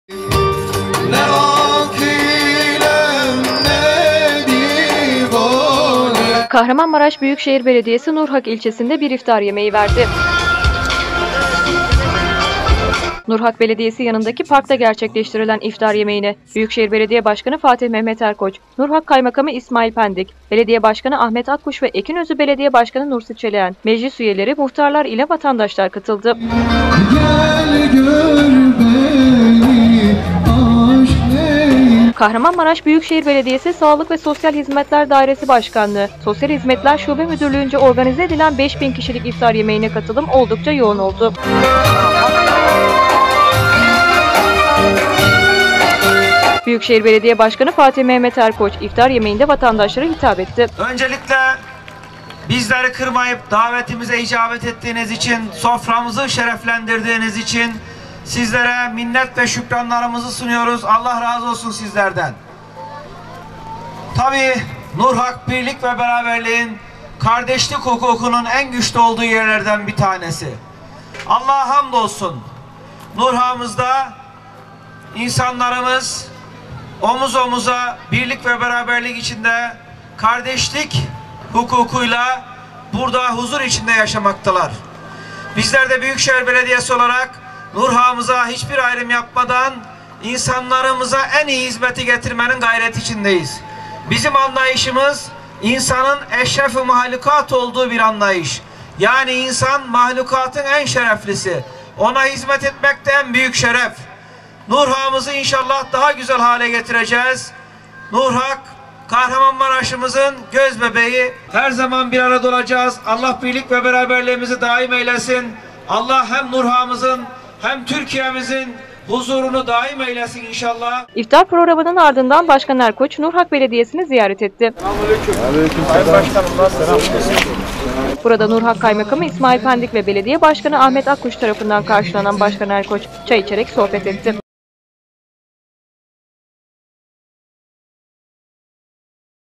Büyükşehir’den Nurhak’ta İftar Yemeği
Kahramanmaraş Büyükşehir Belediyesi Nurhak İlçesi’nde Bir İftar Yemeği Verdi.
Nurhak Belediyesi Yanındaki Parkta Gerçekleştirilen İftar Yemeğine Büyükşehir Belediye Başkanı Fatih Mehmet Erkoç, Nurhak Kaymakamı İsmail Pendik, Belediye Başkanı Ahmet Akkuş Ve Ekinözü Belediye Başkanı Nursi Çeleğen, Meclis Üyeleri, Muhtarlar İle Vatandaşlar Katıldı.
Kahramanmaraş Büyükşehir Belediyesi Salık Ve Sosyal Hizmetler Dairesi Başkanlığı Sosyal Hizmetler Şube Müdürlüğü’nce Organize Edilen 5 Bin Kişilik İftar Yemeğine Katılım Oldukça Yoğun Oldu.
Büyükşehir Belediye Başkanı Fatih Mehmet Erkoç, İftar Yemeğinde Vatandaşlara Hitap Etti.
İftar Programının Ardından Başkan Erkoç Nurhak Belediyesi’ni Ziyaret Etti. Burada Nurhak Kaymakamı İsmail Pendik Ve Belediye Başkanı Ahmet Akkuş Tarafından Karşılanan Başkan Erkoç Çay İçerek Sohbet Etti.